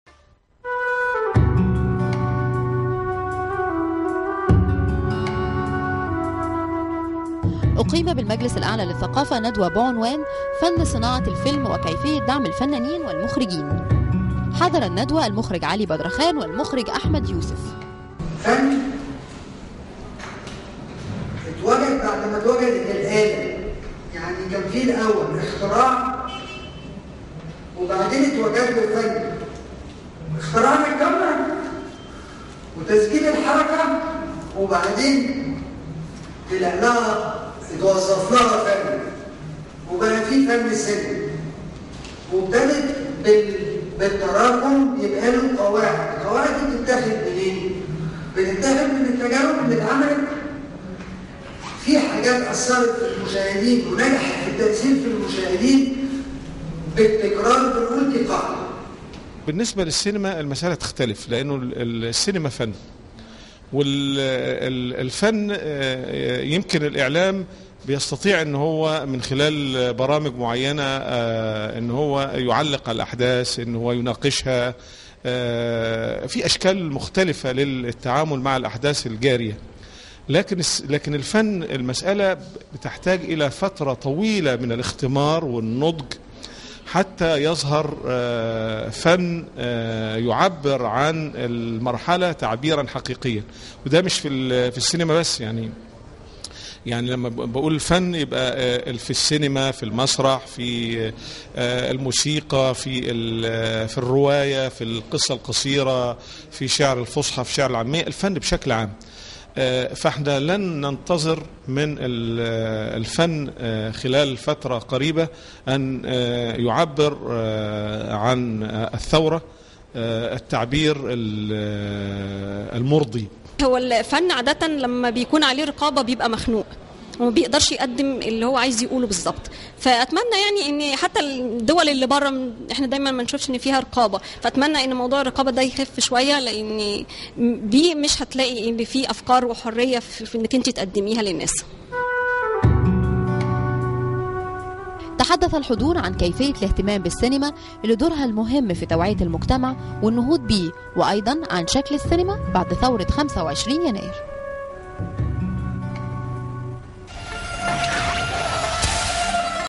دعم فن صناعة السينما
تقرير عن دعم فن صناعة السينما
حلقة الجمعة 21 أكتوبر 2011